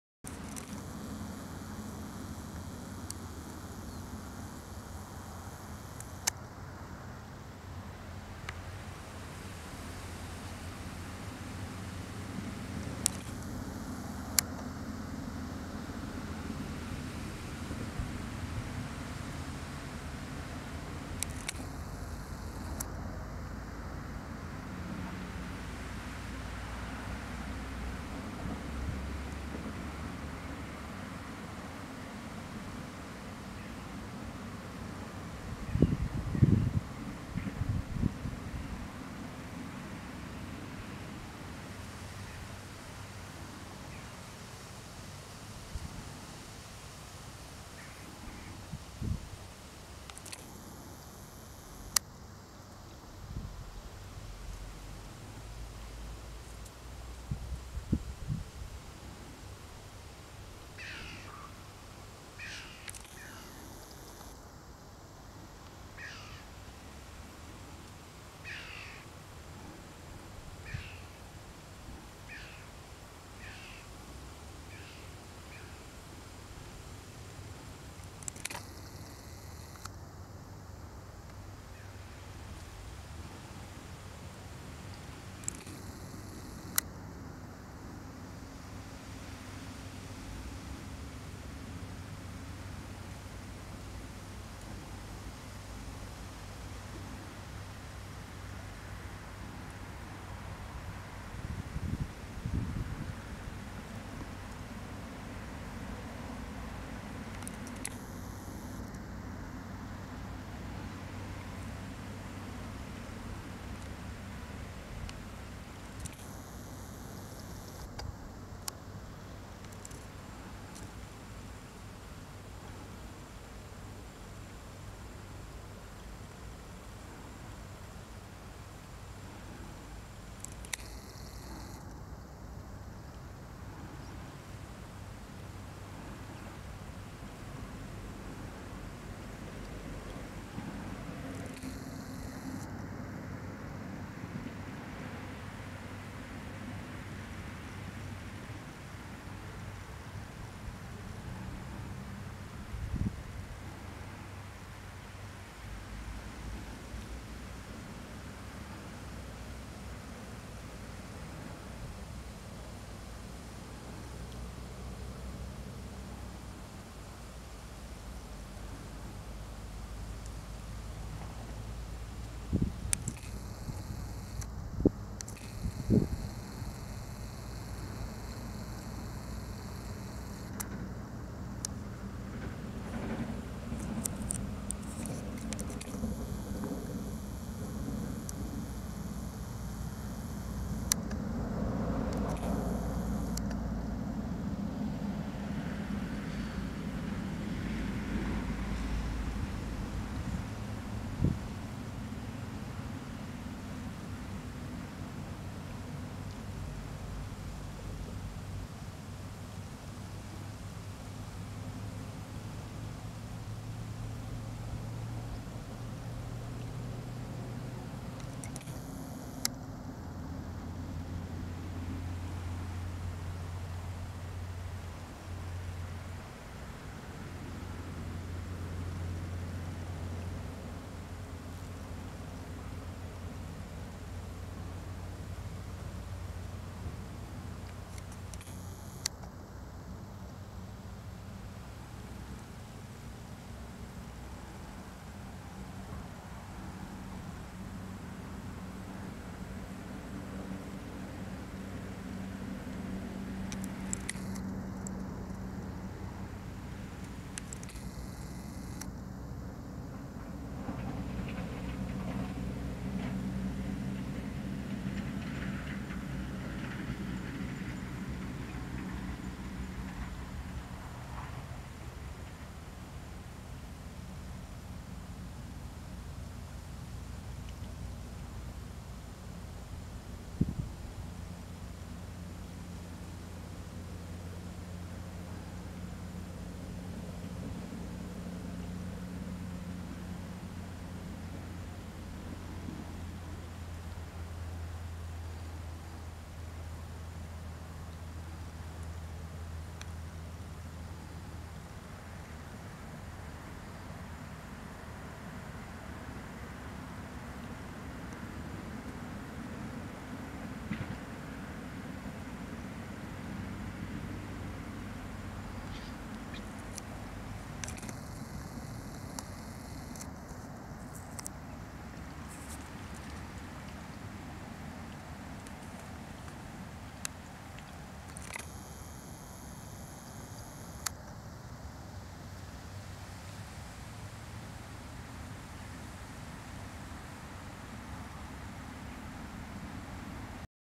как я прикормил уточку
несмотря на то что хлебушек у меня быстро закончился ,она еще долго не хотела уплывать.
видимо хлеб значительно вкусней ,чем тина Рыбинского водохранилища и различная рыбешка.
видео снято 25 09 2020 . Рыбинск Россия